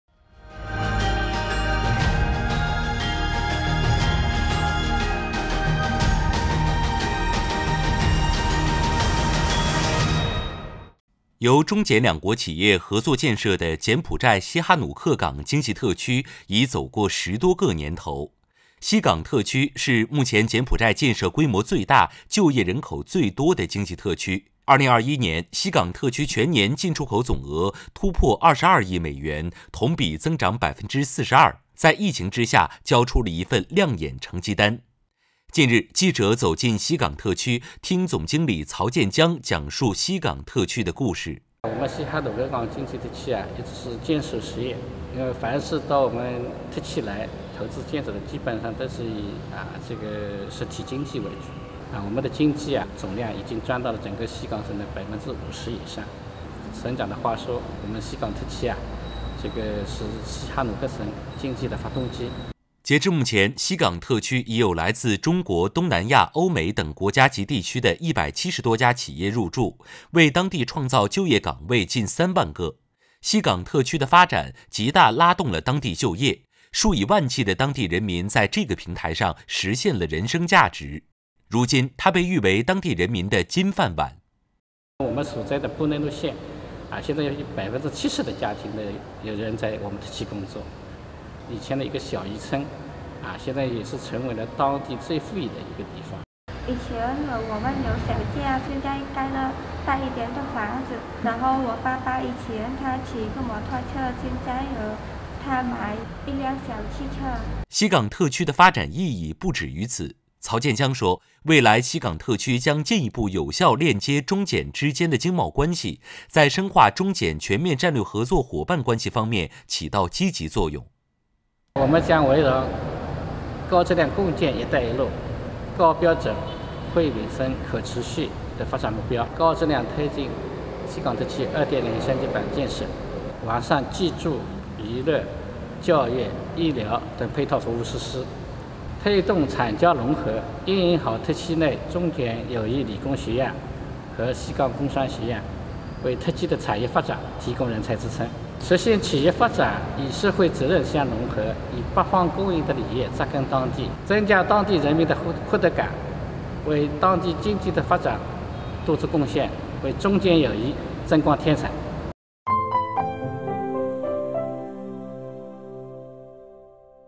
探访西哈努克港经济特区：中柬务实合作的样板
由中柬两国企业合作建设的柬埔寨西哈努克港经济特区（简称西港特区）已走过10多个年头，西港特区是目前柬埔寨建设规模最大、就业人口最多的经济特区。2021年，西港特区全年进出口总额突破22亿美元，同比增长42%，在疫情之下交出了一份亮眼成绩单。近日记者走进西港特区，听总经理曹建江讲述西港特区的故事。

截至目前，西港特区已有来自中国、东南亚、欧美等国家及地区的170多家企业入驻，为当地创造就业岗位近3万个。西港特区的发展极大拉动了当地就业，数以万计的当地人民在这个平台上实现了人生价值。如今，它被誉为当地人民的“金饭碗”。

西港特区的发展意义不止于此。曹德江说，未来西港特区将进一步有效链接中柬之间的经贸关系，在深化中柬全面战略合作伙伴关系方面起到积极作用。